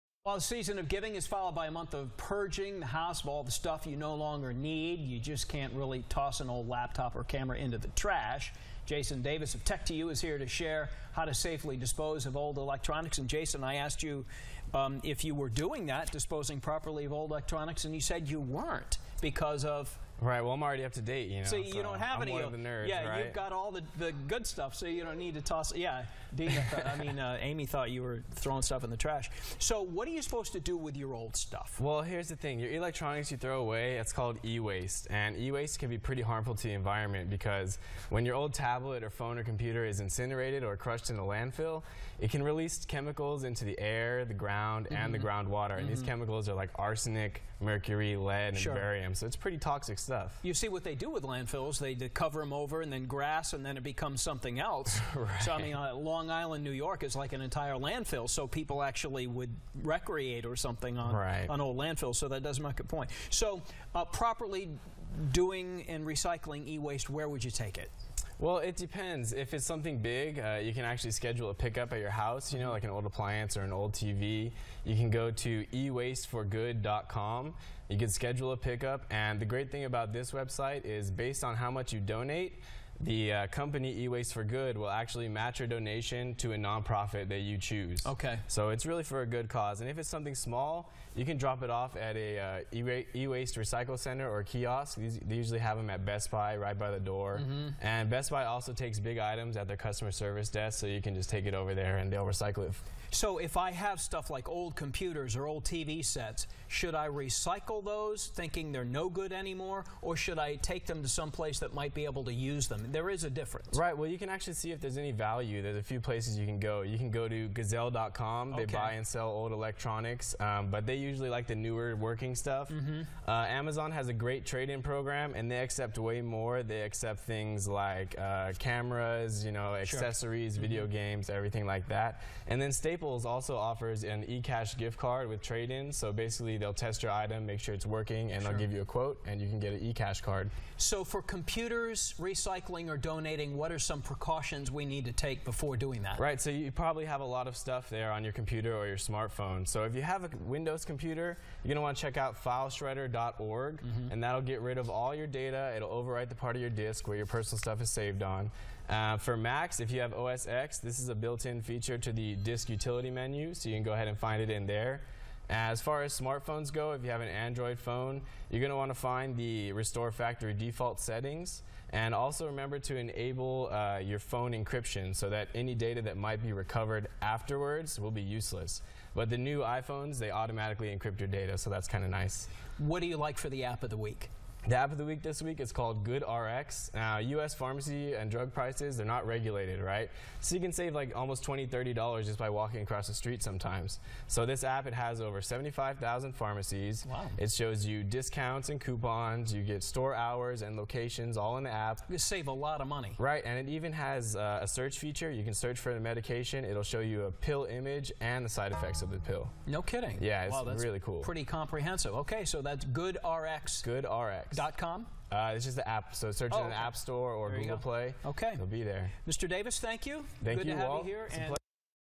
How to Safely Dispose of Old Tech
Jason Davis from Tech2U is with ABC10 Mornings to talk about how to safely dispose of your old tech. You can't just throw an old computer or tablet in the trash; there are a few steps you can take to save the environment from harmful eWaste.